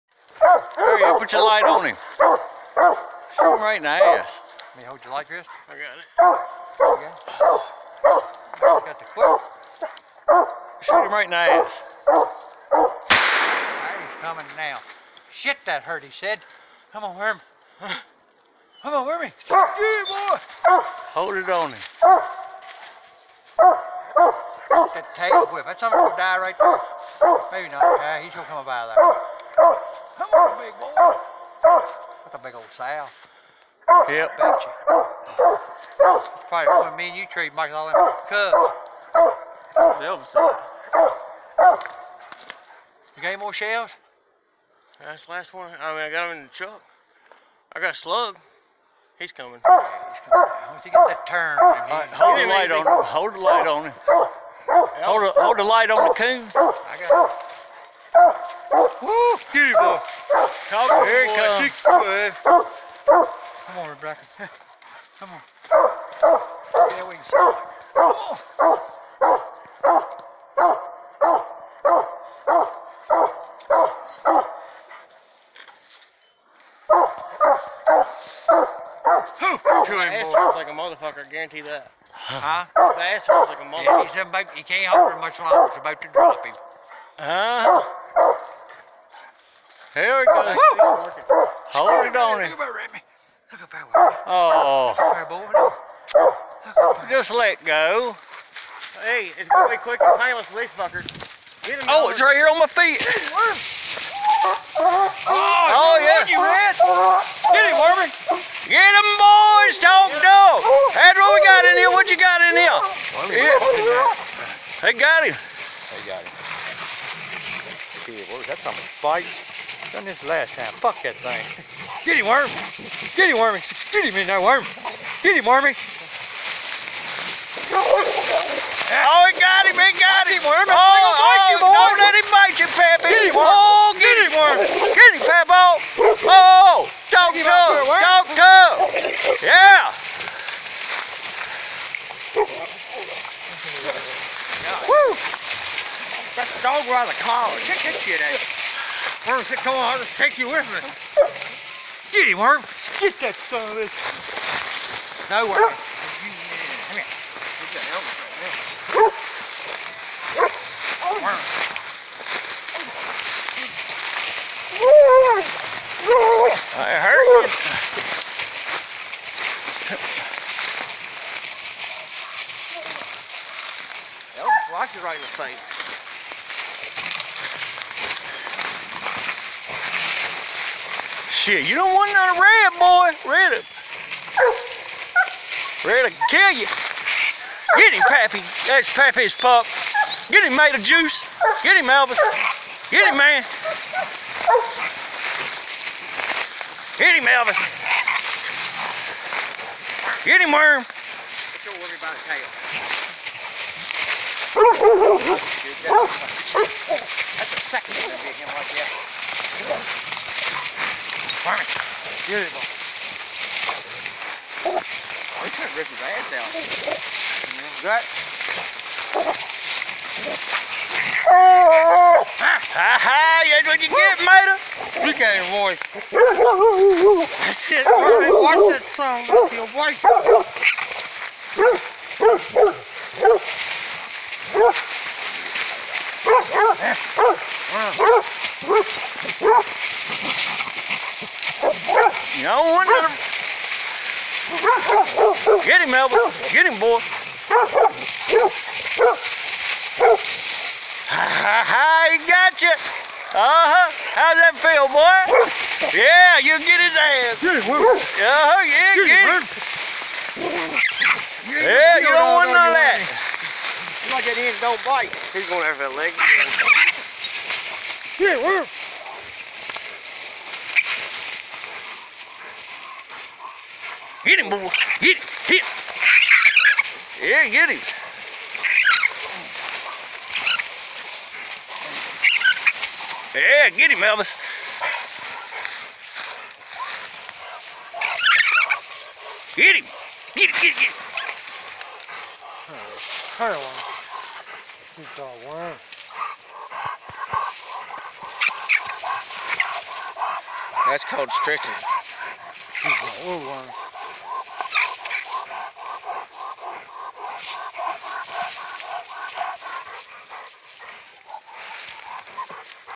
Attack N Hardwood Elvis & Friends Coon Fight
Elvis and friends fight ringtail *excuse the language* and this video may not be suitable for all viewers. Viewer discression is advised.